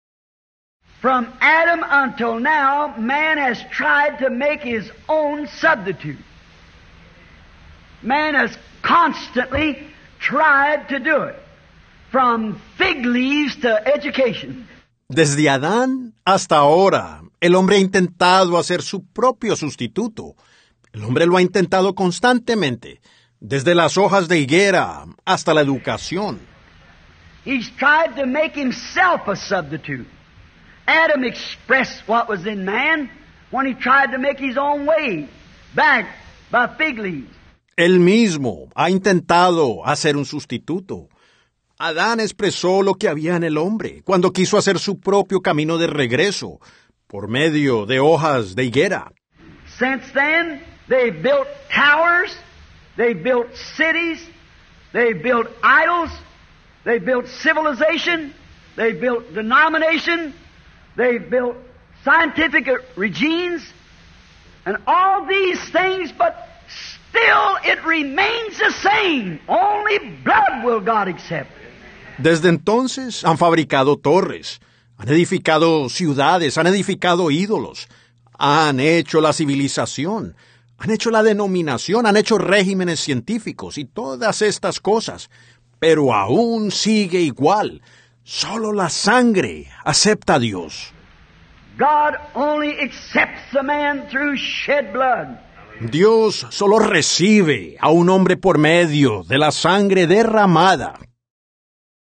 "Todas Las Cosas" Rv. William Branham
62-1124E

Todas Las Cosas

Desde Adán hasta ahora, el hombre ha intentado hacer su propio substituto. El hombre lo ha intentado constantemente, desde las hojas de higuera hasta la educación. Él mismo ha intentado hacer un substituto. Adán expresó lo que había en el hombre, cuando quiso hacer su propio camino de regreso por medio de hojas de higuera. Desde entonces, han fabricado torres, han edificado ciudades, han edificado ídolos, han hecho la civilización, han hecho la denominación, han hecho regímenes científicos y todas estas cosas. Pero aún sigue igual: solo la Sangre acepta Dios.

Dios solo recibe a un hombre por medio de la Sangre derramada.

📖 Pan diario

Hebreos 10:10

En esa voluntad somos santificados mediante la ofrenda del cuerpo de Jesucristo hecha una vez para siempre.